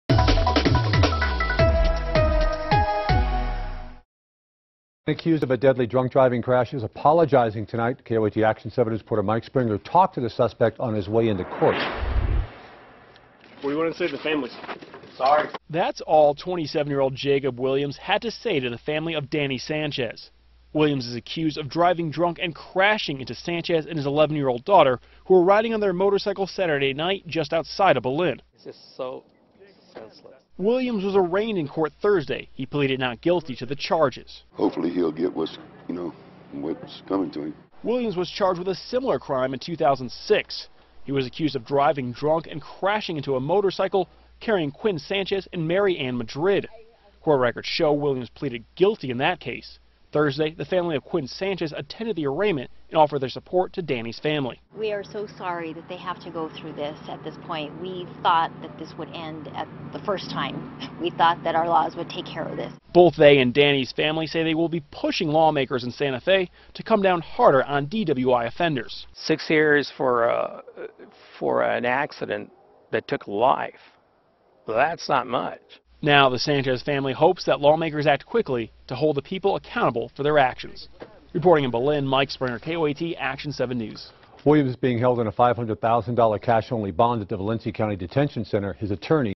Jacob Williams in Court
A man accused of a deadly drunk driving crash is apologizing tonight.